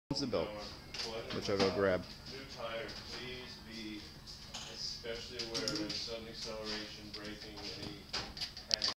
1996 Piaggio Hexagon 2T 5
Let me start off and say Solo Moto in Greenwich Connecticut is the best place to get scooter work done. Next, these vids were shot on April 5 2014, and document work done on my 1996 Piaggio Hexagon scooter. Last, all of these videos are unprocessed. I used a cheap shoot-n-share camcorder that takes horrible video and even worse sound. But you probably just want to see the scooter, right? So who cares? Let me answer with the only correct answer of "no one cares because we want to see an obscure Piaggio Hexagon that some how made it to America". Good answer!